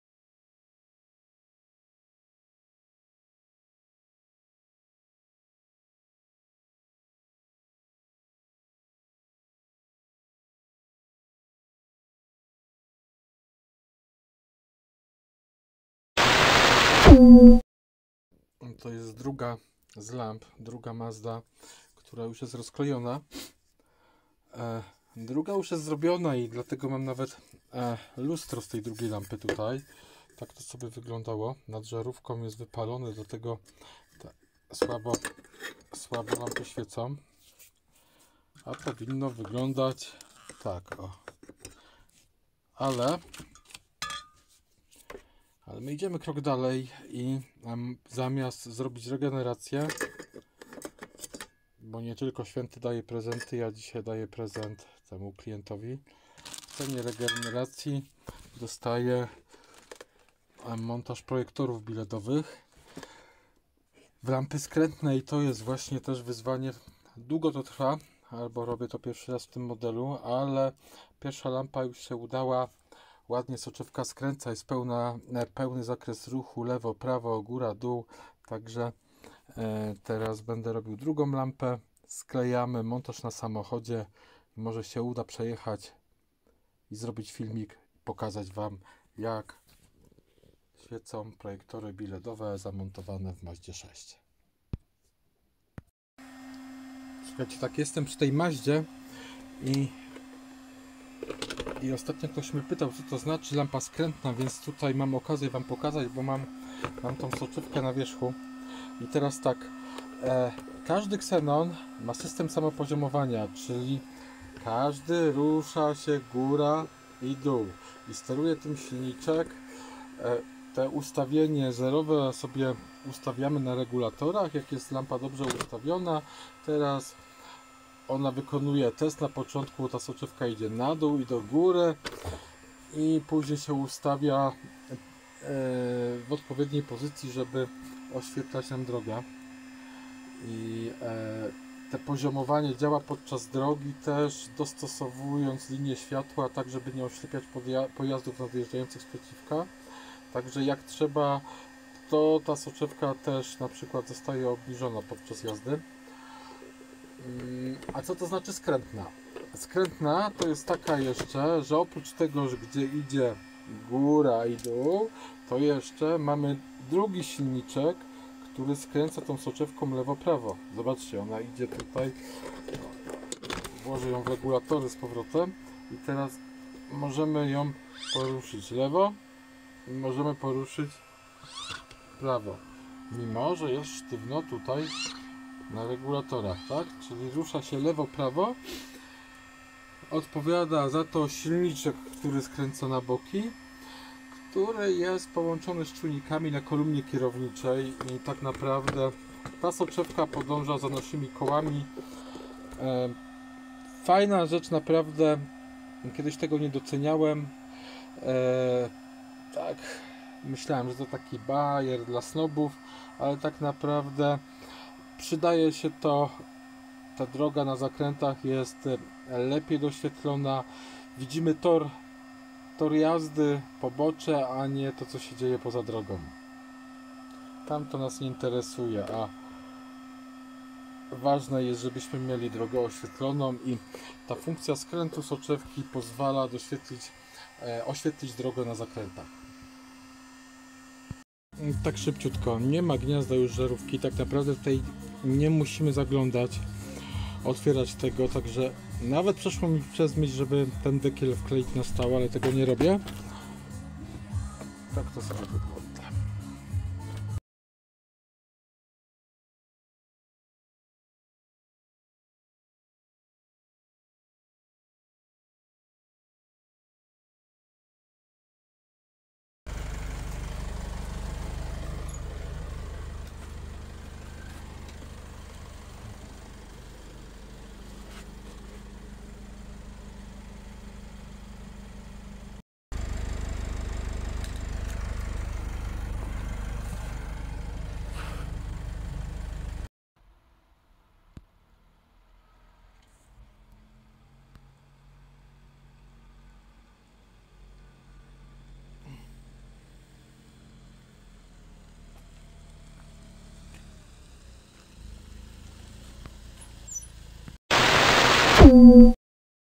Mazda6 lift + montaż soczewek BI-LED w oryginalne lampy e-mail
Lampy w mazda mazda6 coraz słabiej oświetlają drogę przyczyną słabego światła jest wypalona powłoka lustrzana. Taka lampę można zregenerować. Naprawa zajmuje jeden dzień. Po regeneracji lampy świeca jak.
Na życzenie klienta możemy iść o krok do przodu i zamiast regeneracji zamontować projektory BI-LED
Zrealizujemy każdy pomysl
Usługi wykonujemy również wysyłkowo.
Po otrzymaniu paczki zadzwonię aby uzgodnić wszystkie szczegóły. Biorąc pod uwagę czas przesyłek paczka powinna wrocic w ciagu 7-10 dni